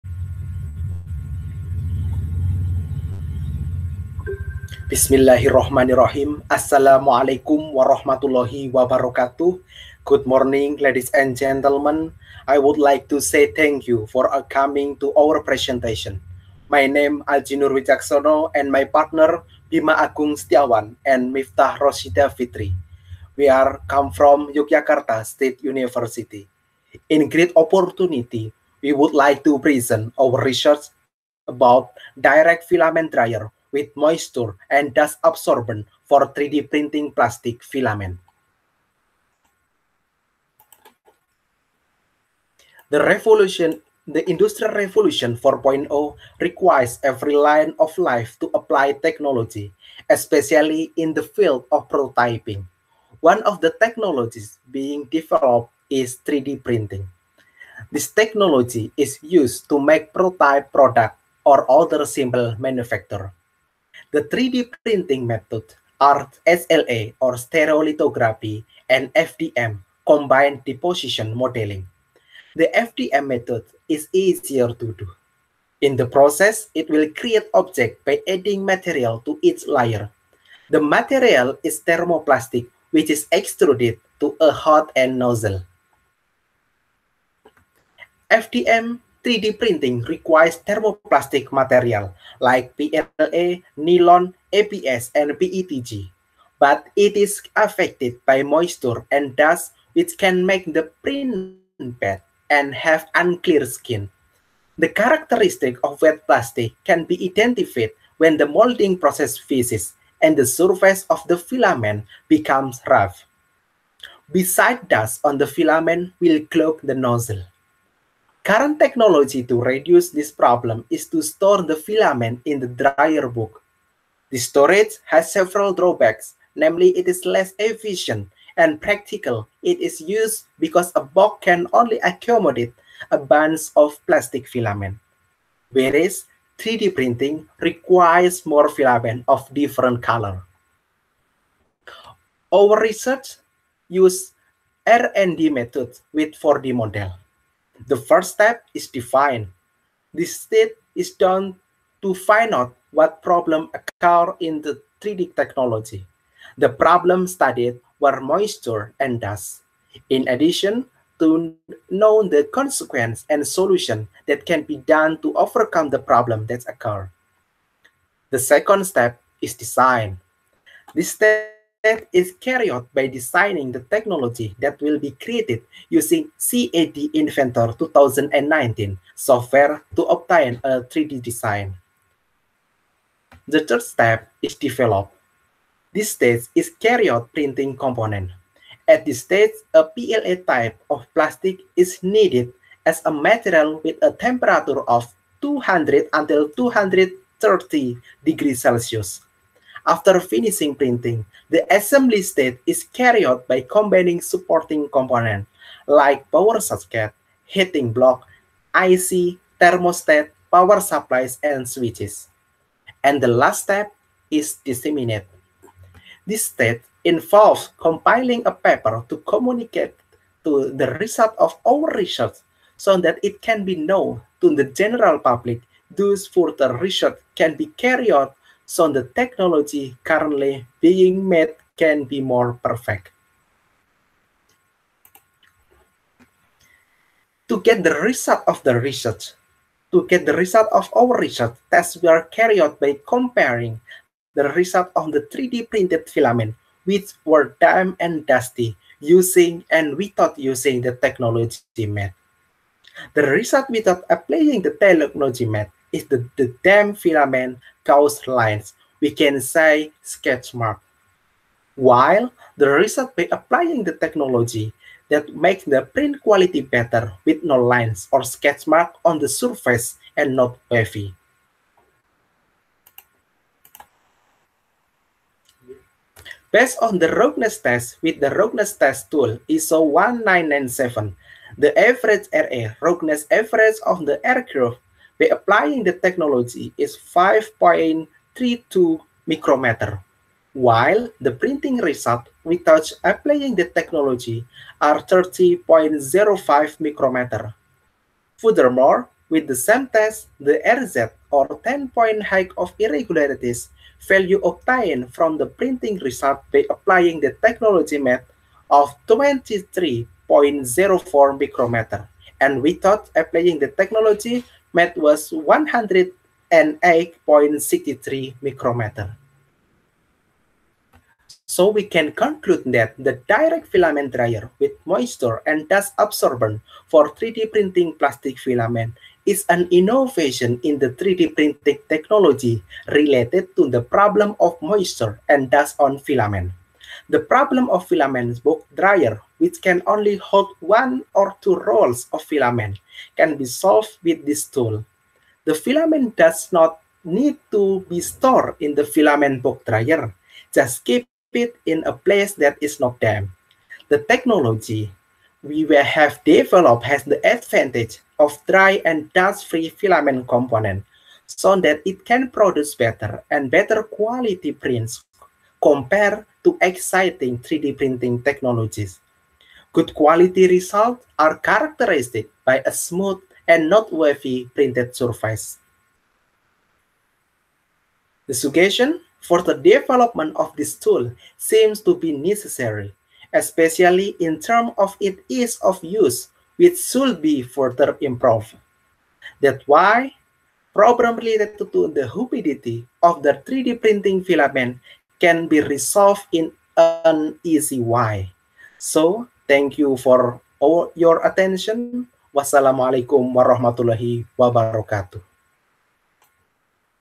Direct Filament Dryer With Moisture And Dust Absorbent For 3D Printing Plastic Filaments
Oral Presentation
Science & Technology - Aji Nur Wijaksono, Bima Agung Setyawan, Miftah Rosyida Fitri